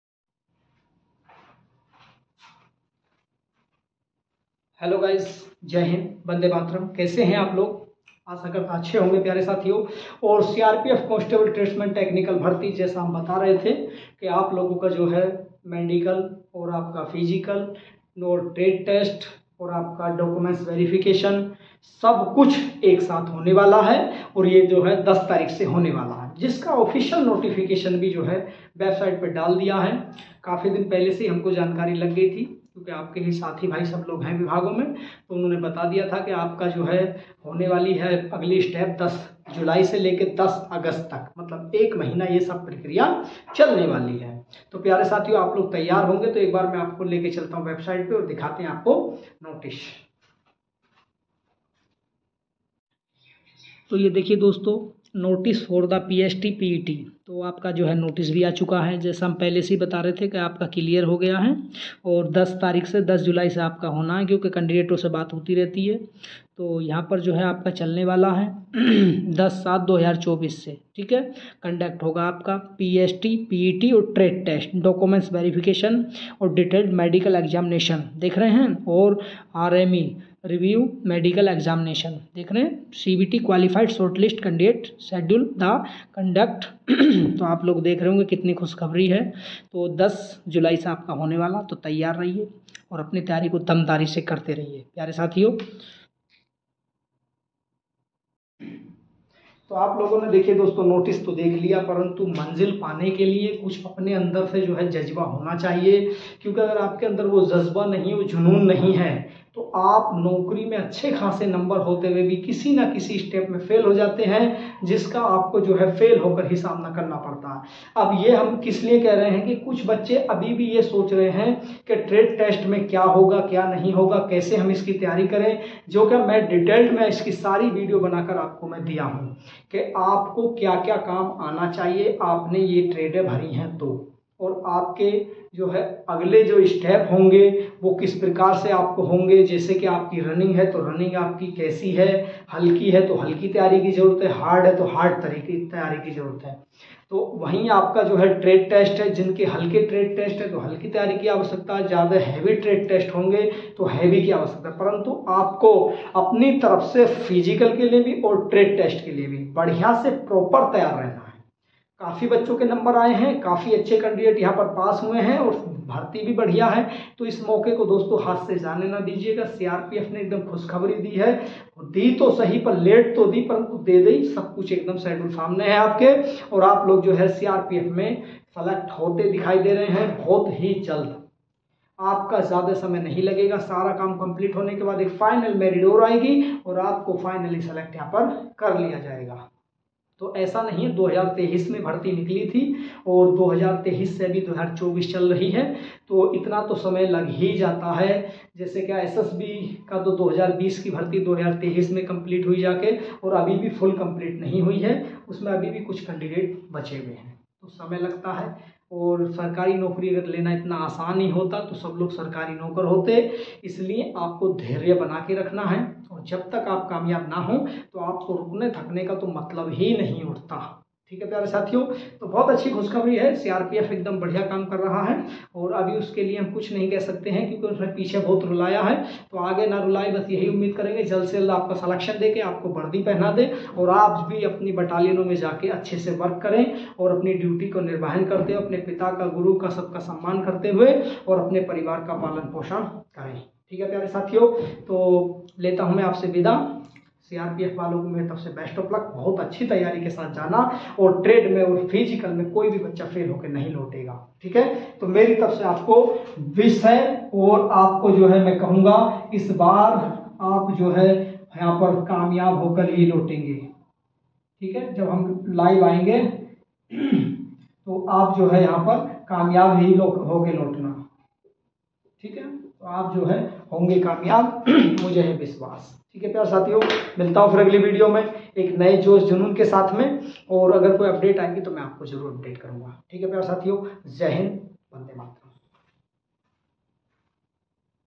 CRPF constable tradesman Bharti 2023 physical DV medical test latest update
CRPF constable tradesman Bharti physical medical test DV sabhi ke liye ho jao taiyar 10 July se